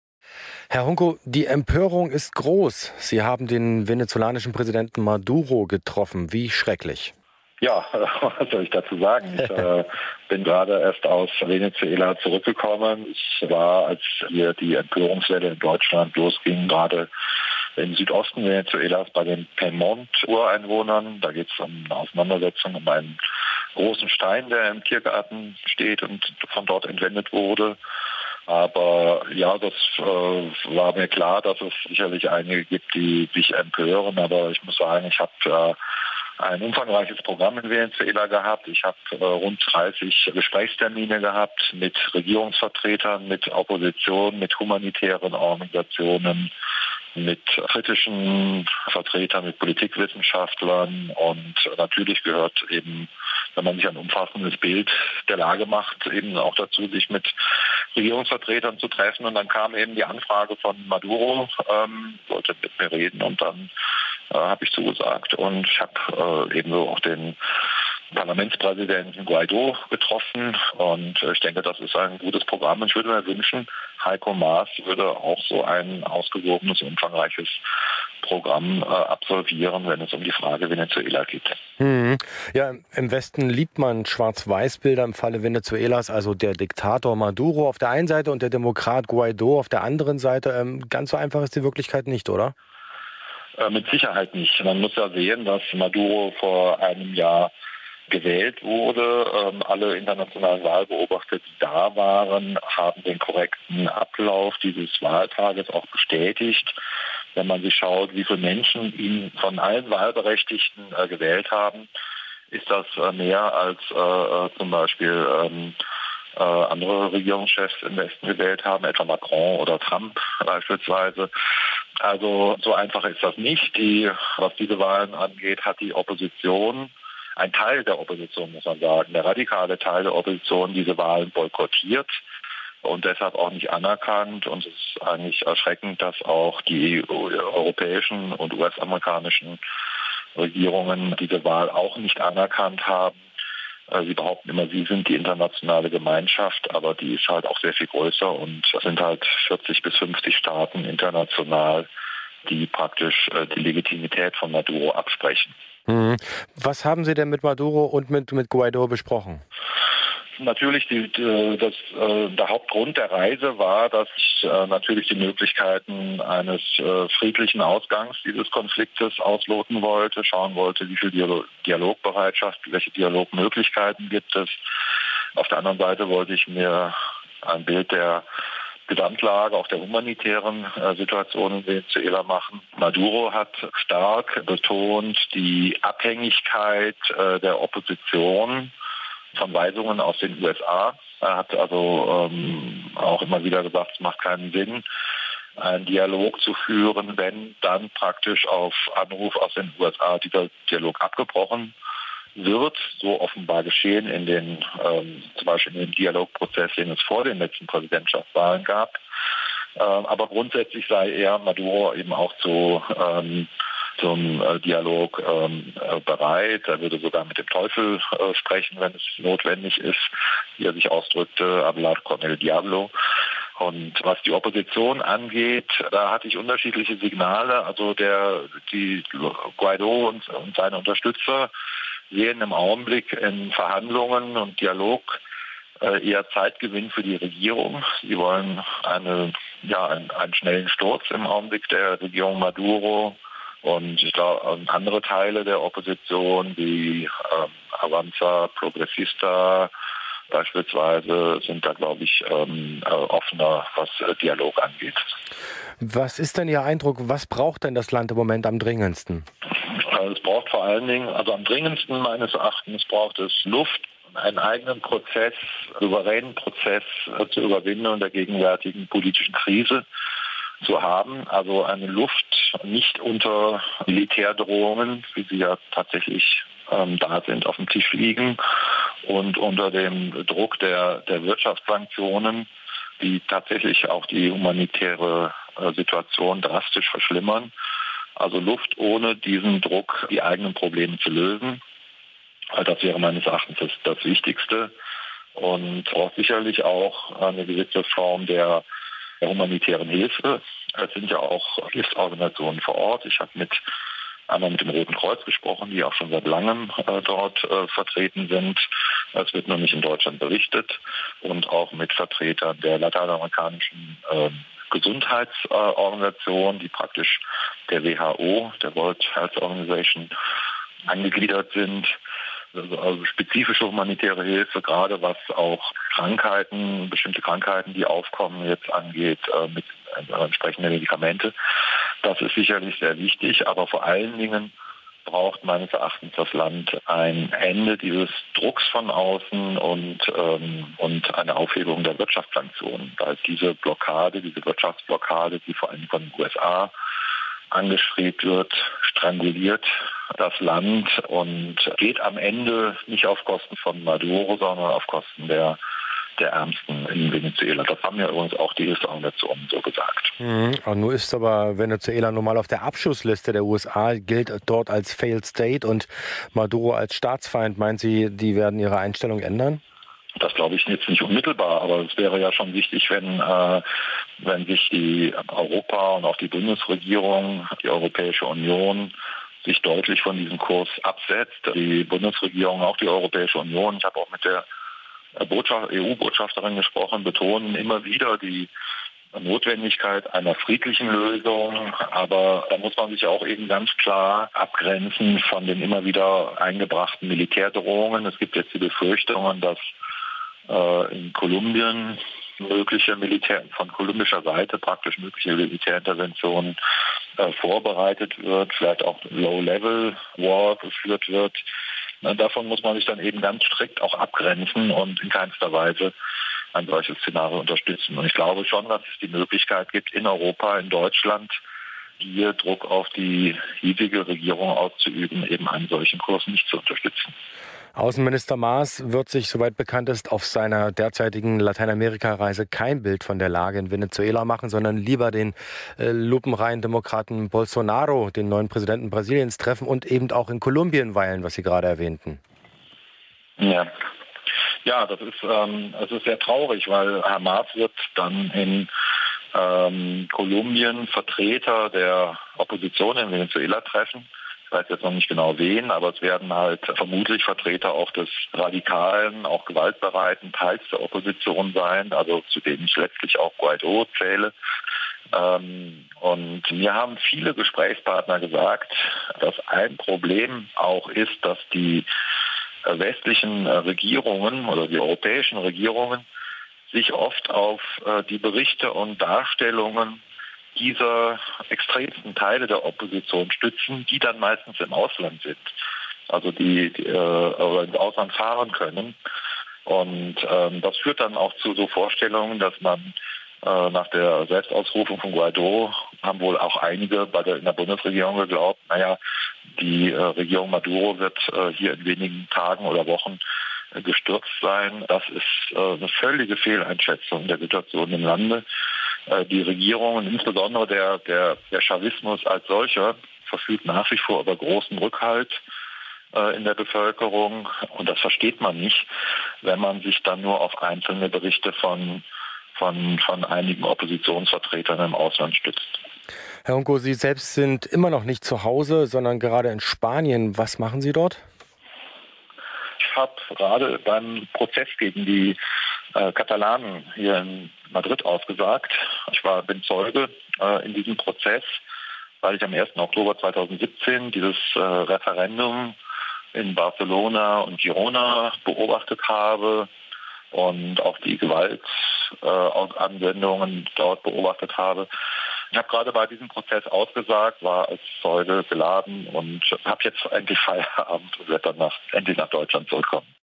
Andrej Hunko (Linke) trifft in Venezuela Maduro und Guaido / Heftige Kritik (Sputniknews)
Übernommener Beitrag von Sputniknews